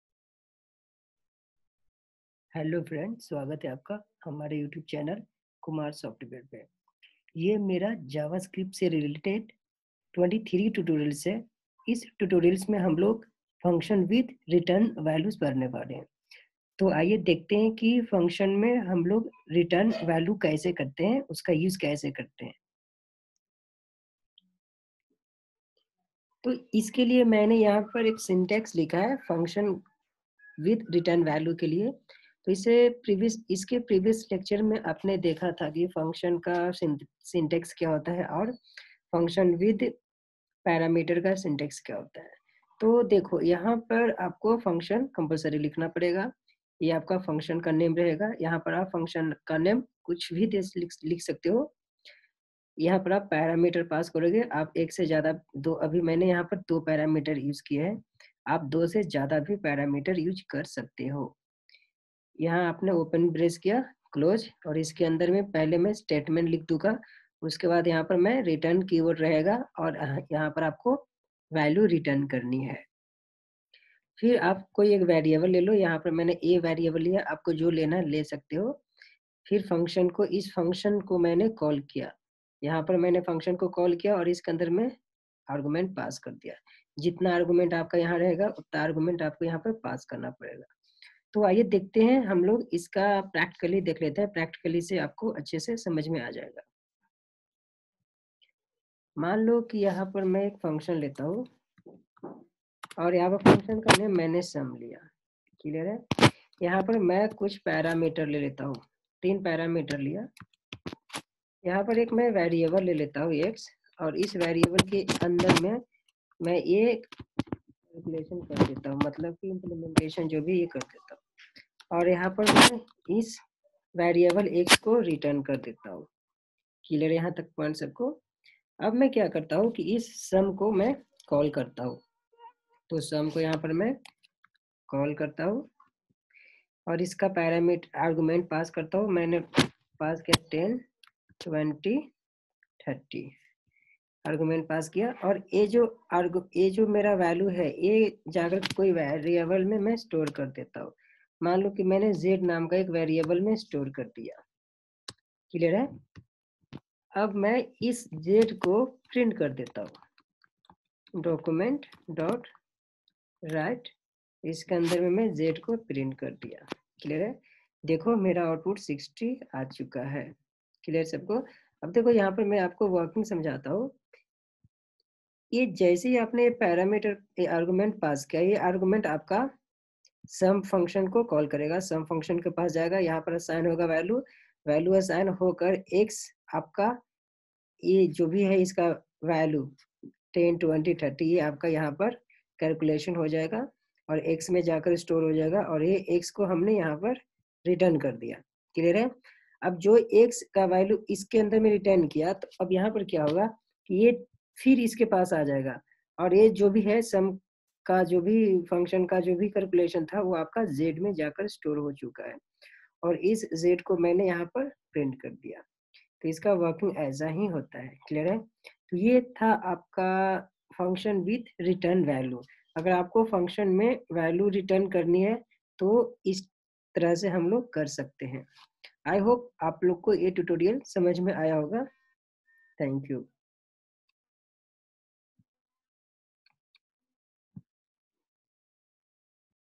JavaScript Functions With Return Value ( Lecture-23 ) in hindi.
Hello Friends, in this tutorial we will learn about how to use return value in function.